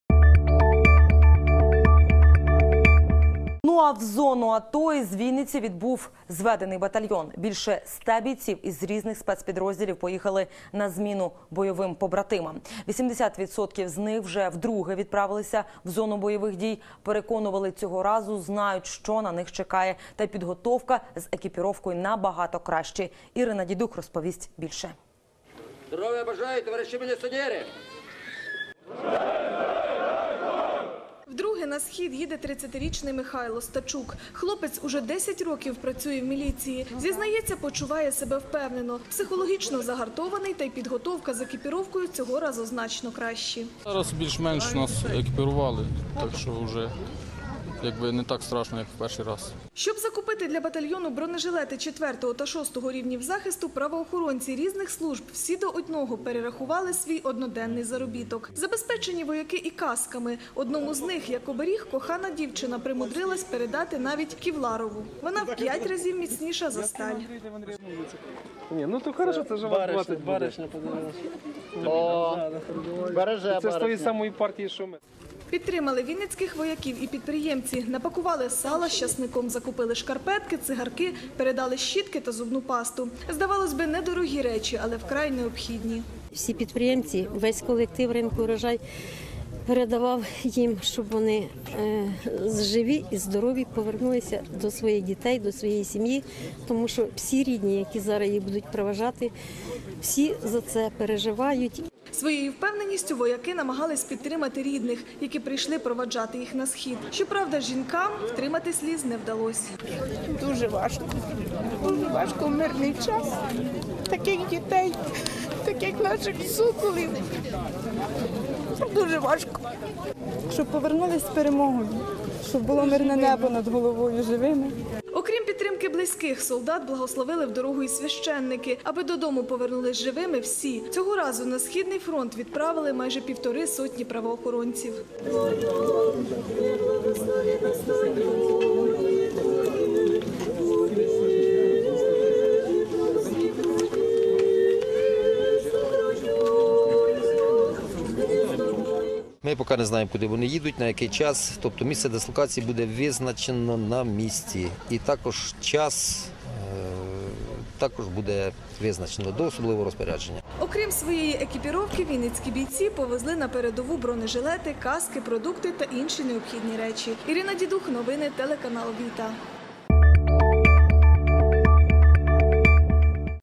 Телеканал ВІТА новини 2014-07-07 Вінничани- знову на схід
В зону АТО з Вінниці відбув зведений батальйон. Більше ста бійців з різних спецпідрозділів поїхали на зміну бойовим побратимам. 80 відсотків з них вже вдруге відправились в зону бойових дій. Переконували, цього разу  знають, що на них чекає та й підготовка з екіпіровкою набагато кращі.